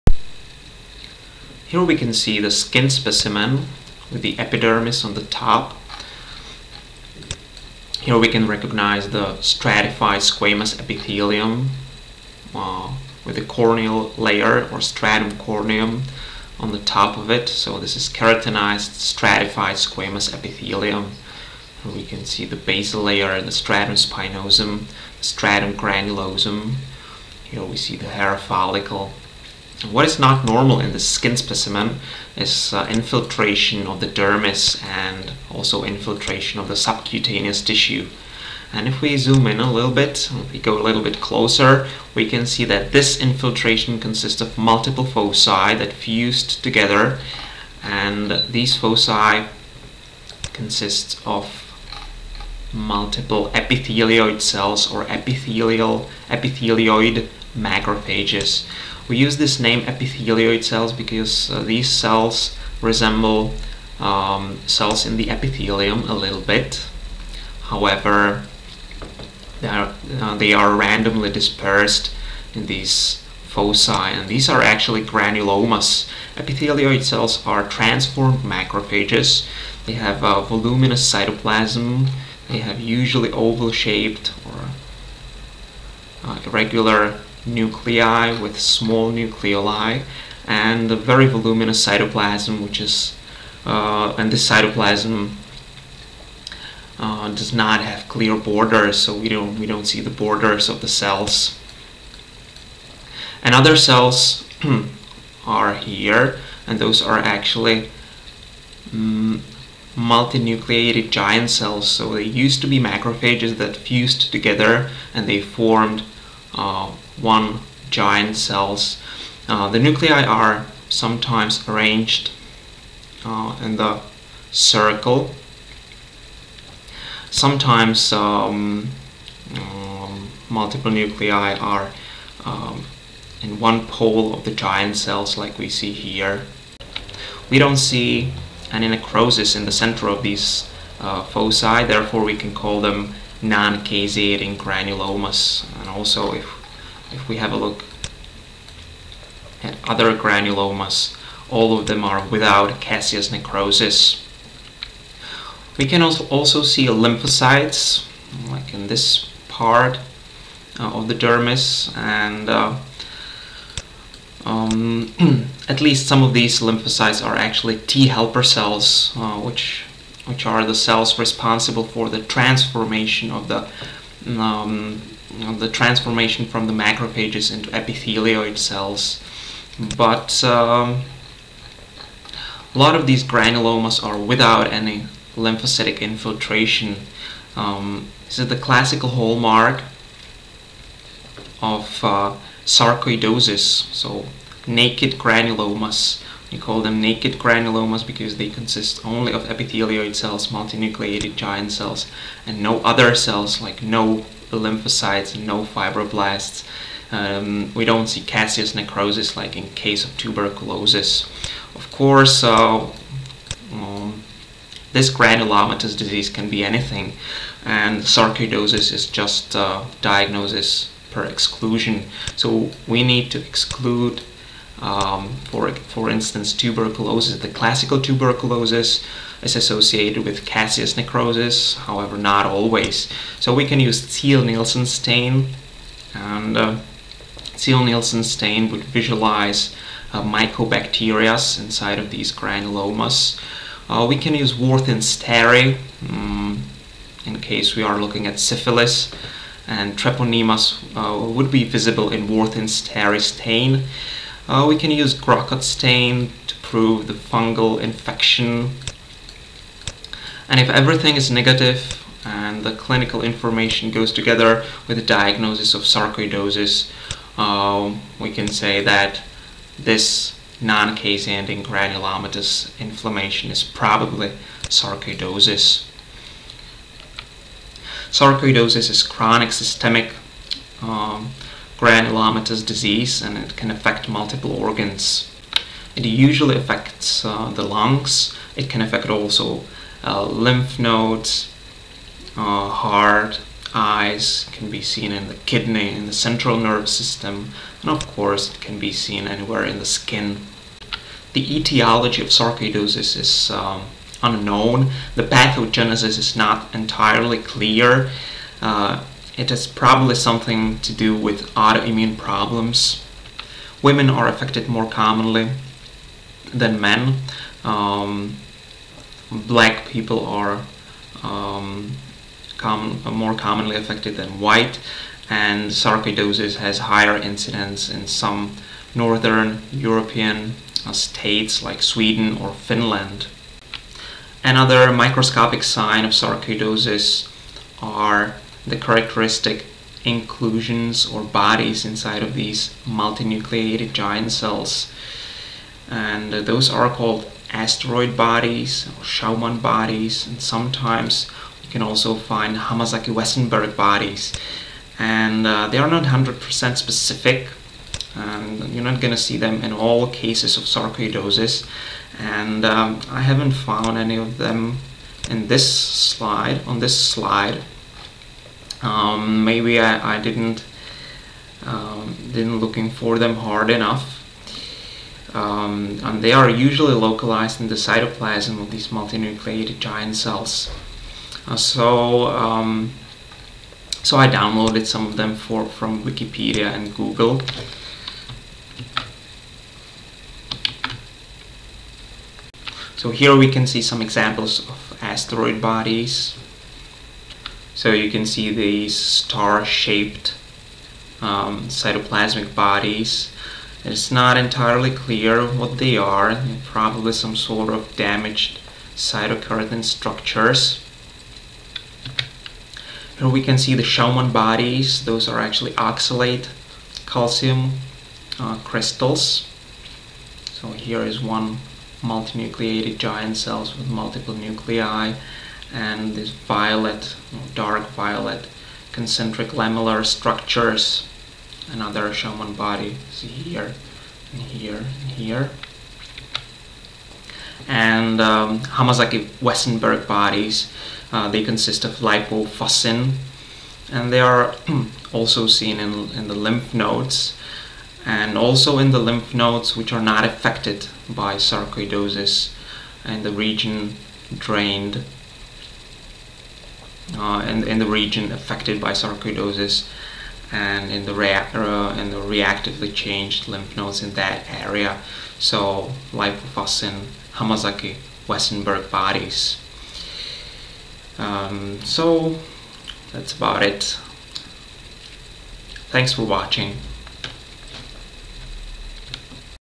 Histopathology - Sarcoidosis of the Skin
Histopathology slides, Slides for the practical part of the pathology exam in Masaryk University.

Sarcoidosis is systemic chronic granulomatous disease
It commonly affects the lungs, lymph nodes and skin.
Any organ can be affected.
Etiology is unknown, probably autoimmune.
Microscopic: non-caseating granulomas, naked granulomas, characteristic inclusions: asteroid bodies, schaumann bodies, Hamazaki-Wesenberg bodies  (not very sensitive, not 100% specific).
Treatment: corticosteroids